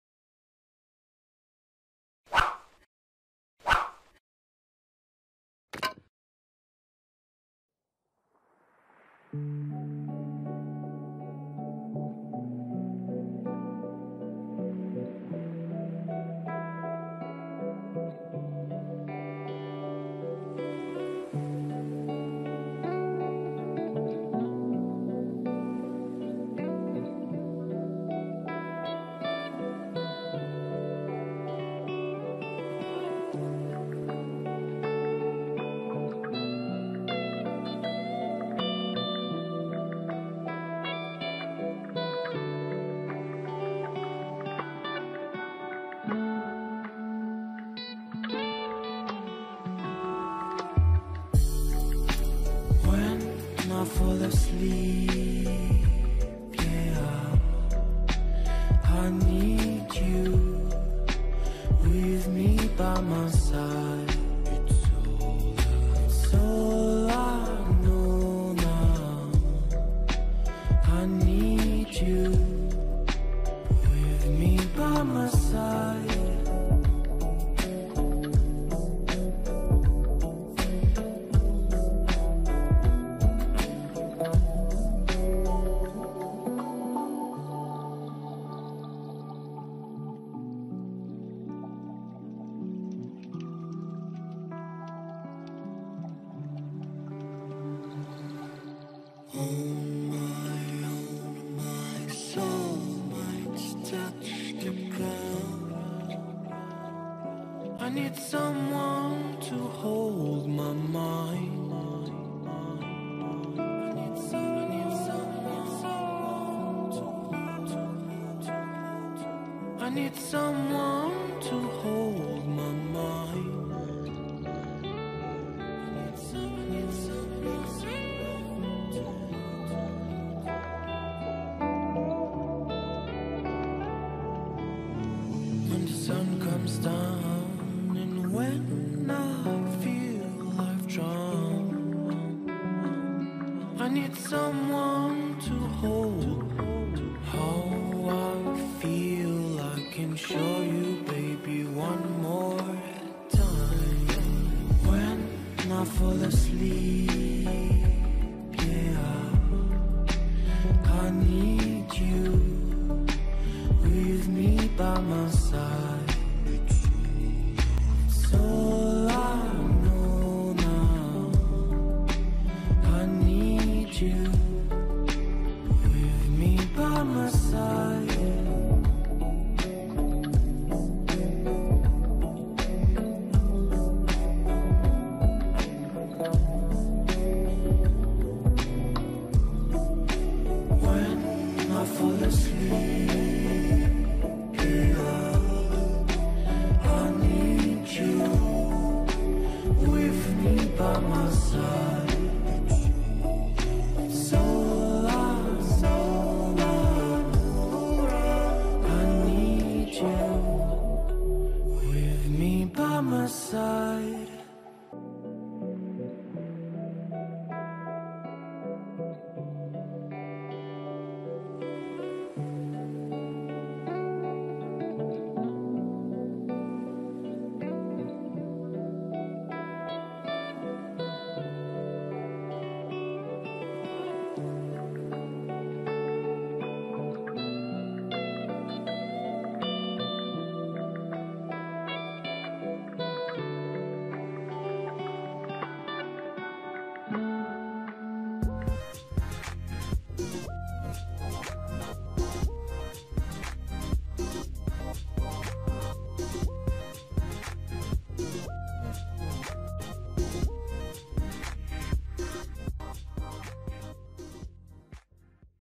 Fish drawing from leaf shape - Step by step drawing tutorial
In this video, I will show you how to draw a Fish from a leaf shape outline in a few minutes.

Used Materials:
1. Surface – White Art Paper
2. Marker – GXin CD/DVD Marker
3. Pencil – Matador HB Pencil & Link Color Pencils